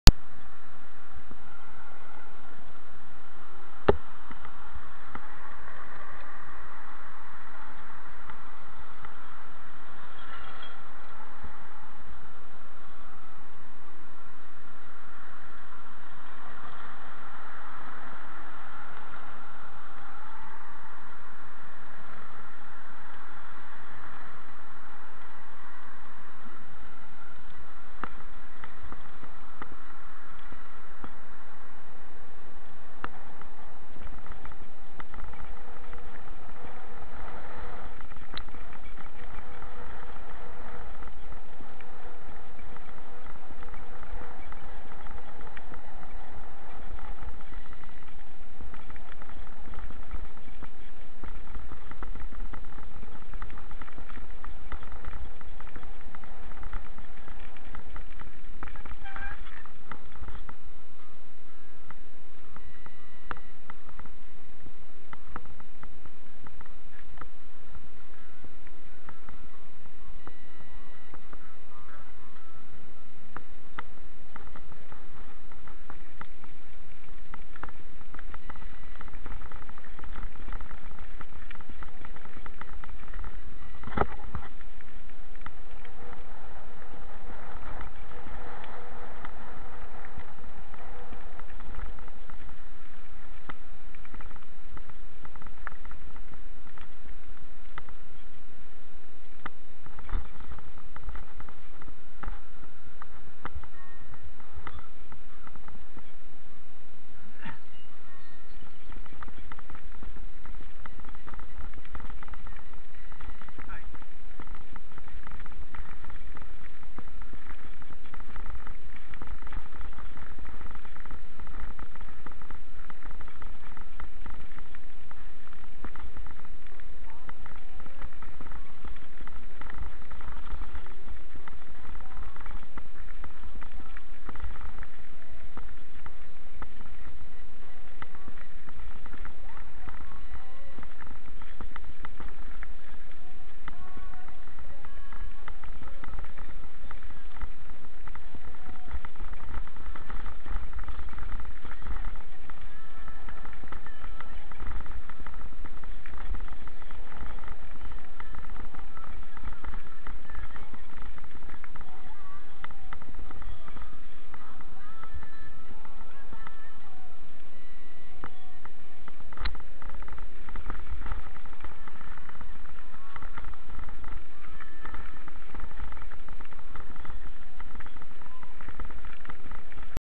riding alongside streetcar tracks
leaving the Southside where the high level streetcar starts, I decided to ride back along the tracks to 109st bridge,  this is in Edmonton, Alberta